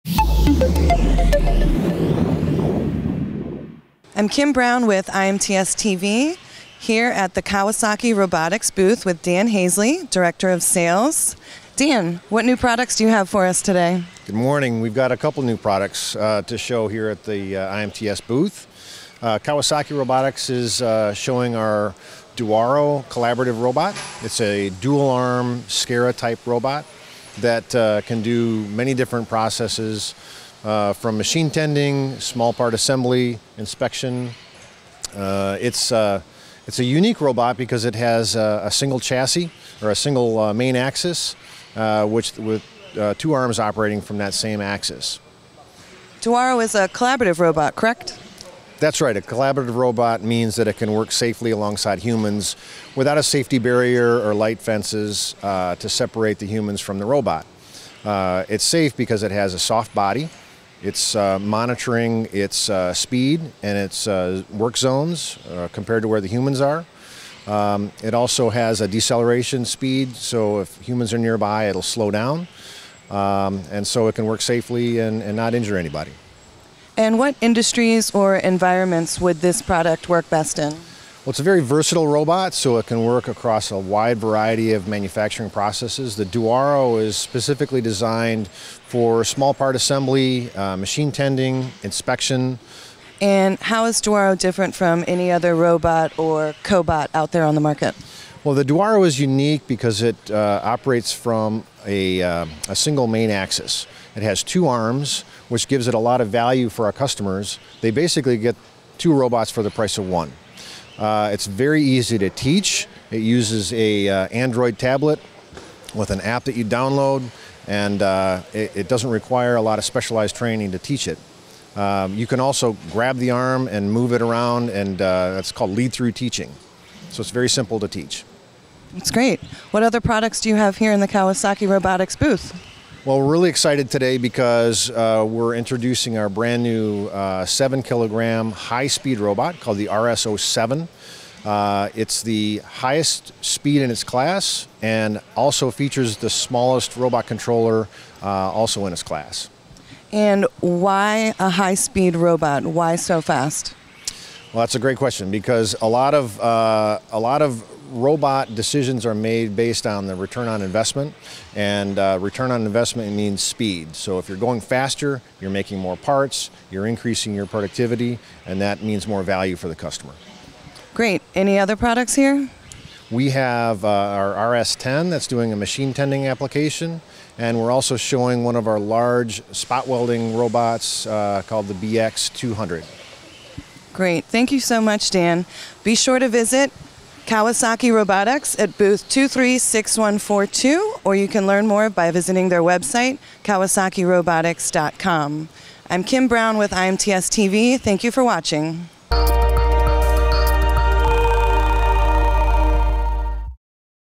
Meet duAro, a Kawasaki Cobot - IMTS 2018
The duAro cobot is the first dual-armed horizontal articulated robot to operate on a single axis, enabling the robot to easily perform coordinated movements, much like a human, making it perfect for applications such as small-part inspection, assembly, material handling, material removal and machine tending. Designed to fit into a single-person space, it can easily be deployed without modifications to any assembly or manufacturing line and keep its human co-workers safe. Low-power motors, a soft body, speed and work zone monitoring, and a deceleration function allows the duAro to safely collaborate with humans in work operations. The direct teach function allows for the user to teach the robot tasks by hand guiding its arms. The robot can also be programmed through an intuitive tablet terminal by entering numerical values indicating the direction and distance of each movement. IMTS 2018 Exhibitor Product Innovation Honorable Mention.